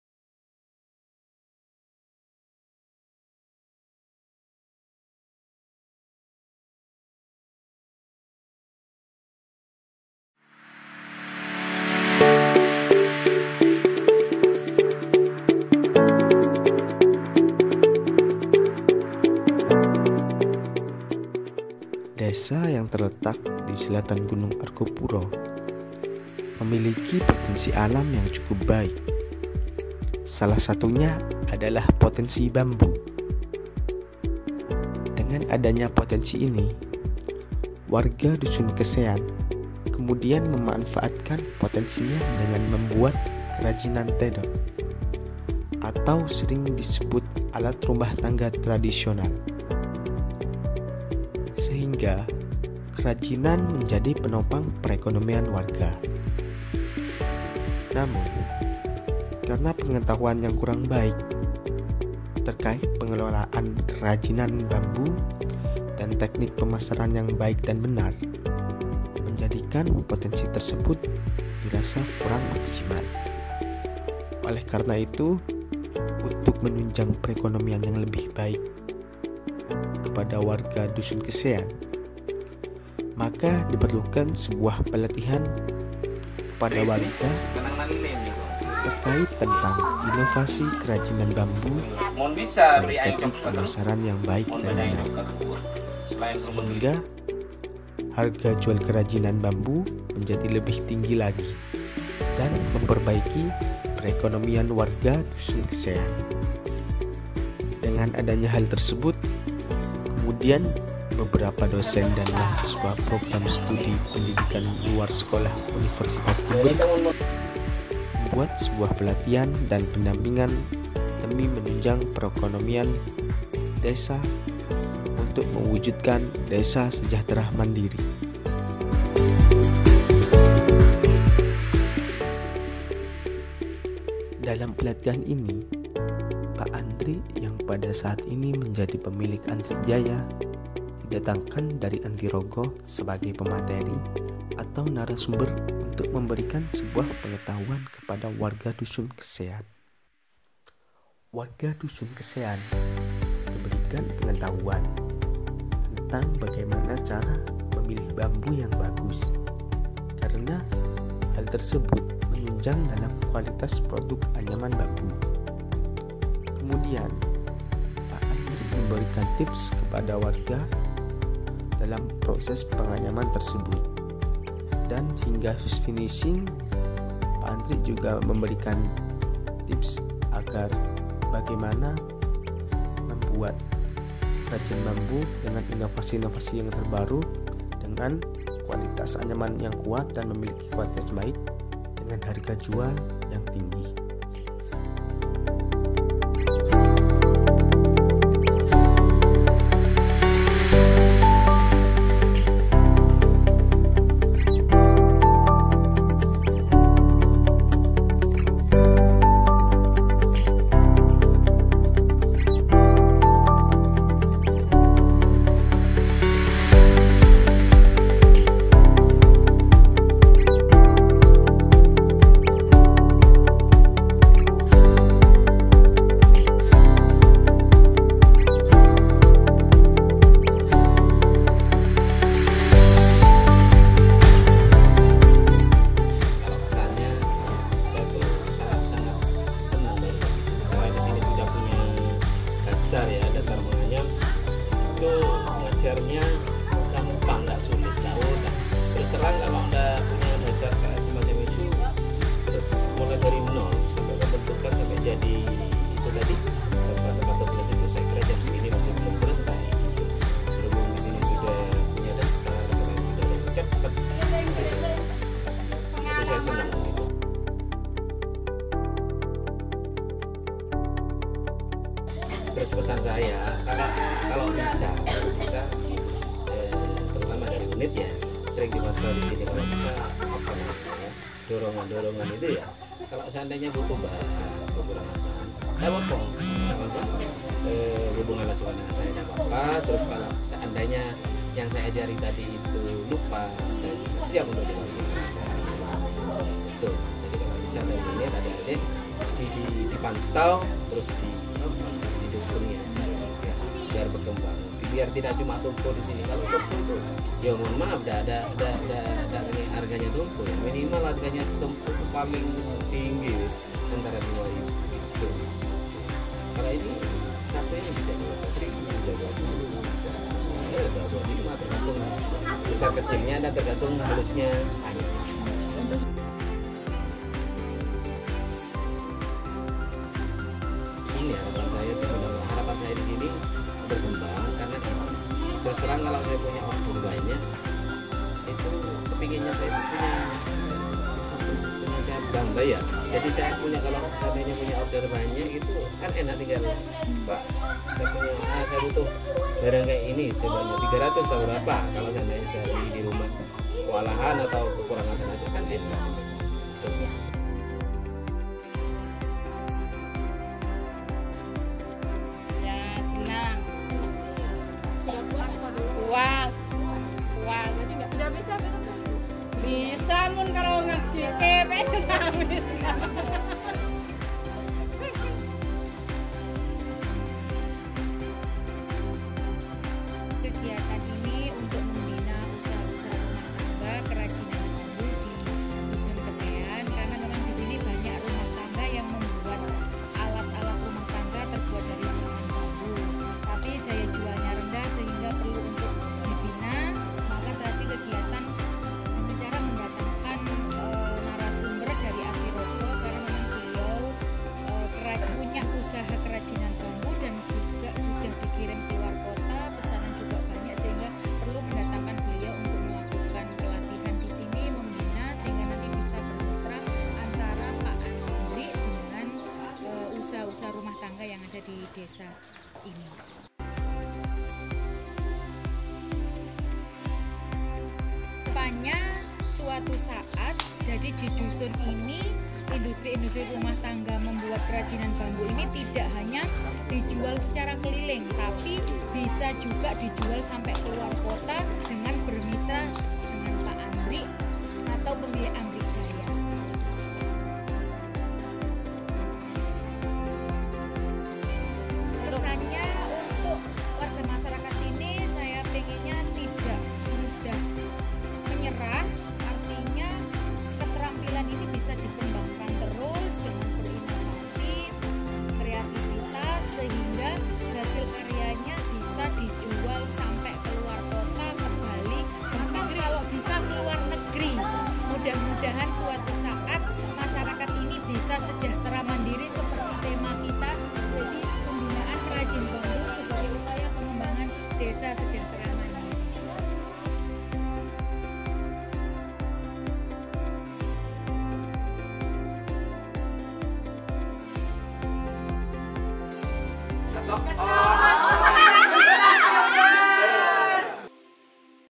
PEMBINAAN USAHA MIKRO ‘PERAJIN BAMBU’ SEBAGAI UPAYA PENGEMBANGAN DESA SEJAHTERA MANDIRI
Pembinaan Usaha Mikro “Perajin Bambu” Sebagai Upaya
Pengembangan Desa Sejahtera Mandiri Di Desa Pakis
Kecamatan Panti Kabupaten Jember. Tim pelaksana pengabdian Unej  Dr. AT.
Hendrawijaya., SH., M.Kes, Linda Fajarwati., S.Pd., M.Pd, Irliana Faiqotul Himmah., S.Pd., M.Pd